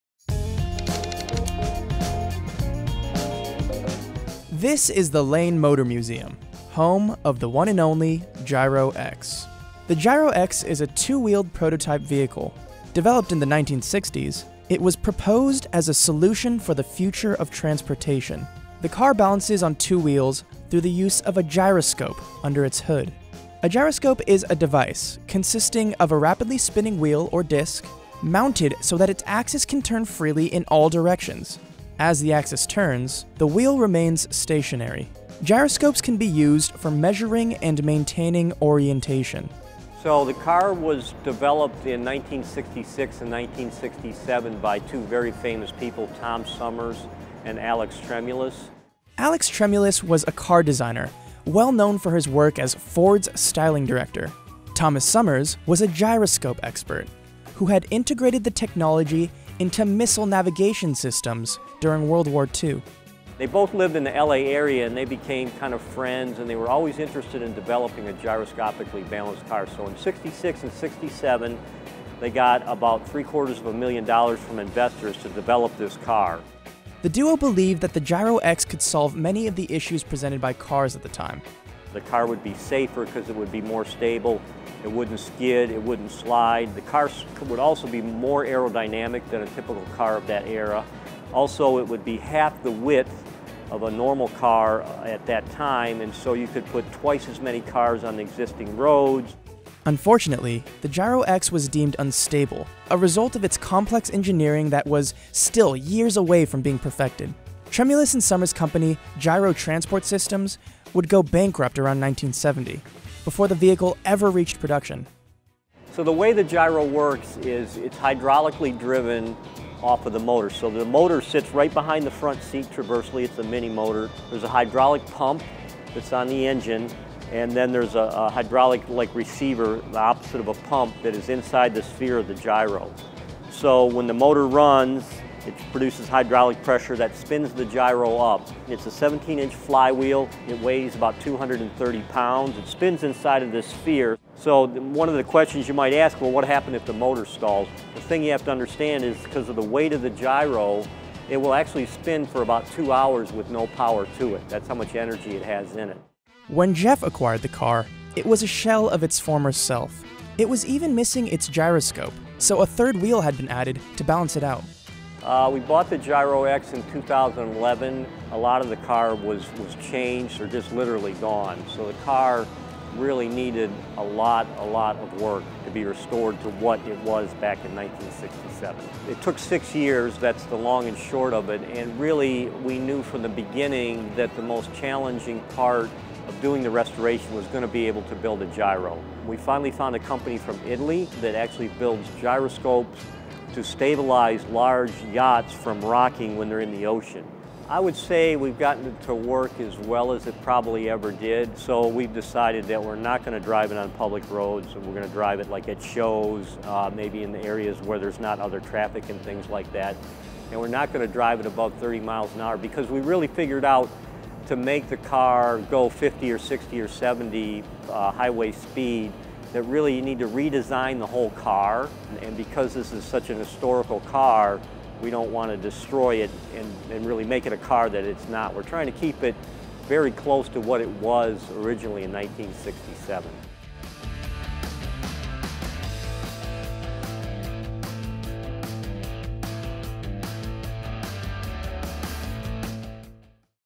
How This Two-Wheeled Car Uses A Disk To Balance
The Lane Motor Museum is the home to some whacky cars. One of the craziest is the Gyro-X that balances on two wheels through the use of a gyroscope.

Following is a transcript of the video:

This is the Lane Motor Museum, home of the one and only Gyro-X. The Gyro-X is a two-wheeled prototype vehicle. Developed in the 1960s, it was proposed as a solution for the future of transportation. The car balances on two wheels through the use of a gyroscope under its hood. A gyroscope is a device consisting of a rapidly spinning wheel or disk mounted so that its axis can turn freely in all directions. As the axis turns the wheel remains stationary. Gyroscopes can be used for measuring and maintaining orientation.

Jeff Lane: So the car was developed in 1966 and 1967 by two very famous people, Tom Summers and Alex Tremulis.

Alex Tremulis was a car designer well known for his work as Ford’s styling director. Thomas Summers was a gyroscope expert who had integrated the technology into missile navigation systems during World War II.

Jeff Lane: They both lived in the LA area and they became kind of friends. And they were always interested in developing a gyroscopically balanced car. So in '66 and '67 they got about three quarters of a million dollars together from investors to develop this car.

The duo believed the Gyro-X could solve many of the issues presented by cars at the time.

Jeff Lane: The car would be safer because it would be more stable. It wouldn't skid. It wouldn't slide. The car would also be more aerodynamic than a typical car of that era. Also it would be half the width of a normal car at that time and so you could put twice as many cars on existing roads.

Unfortunately the Gyro-X was deemed unstable, a result of its complex engineering that was still years away from being perfected. Tremulis and Summers’ company Gyro Transport Systems would go bankrupt around 1970 before the vehicle ever reached production.

Jeff Lane: So the way the gyro works is it's hydraulically driven off of the motor. So the motor sits right behind the front seat traversely. It's a mini-motor. There's a hydraulic pump that's on the engine. Then there is a hydraulic pump inside the sphere of the gyro. So when the motor runs it produces hydraulic pressure that spins the gyro up. It's a 17-inch flywheel. It weighs about 230 pounds. It spins inside of this sphere.

When Jeff acquired the car, it was a shell of its former self. It was even missing its gyroscope, so a third wheel had been added to balance it out.

Jeff Lane: We bought the car in 2011. A lot of the car was changed or just literally gone. So the car really needed a lot of work to be restored to what it was in 1967. It took six years. We knew from the beginning the most challenging part of doing the restoration would be building a gyro. We finally found a company from Italy that builds gyroscopes to stabilize large yachts from rocking when they're on the ocean. I would say we've gotten it to work as well as it ever did. So we've decided we're not going to drive it on public roads and we're not going to drive it over 30 mph because we've figured out that to make the car go highway speeds you have to redesign the whole car. And because this is such a historic car we don't want to destroy it and make it a car that it's not. We're trying to keep it close to what it was originally in 1967.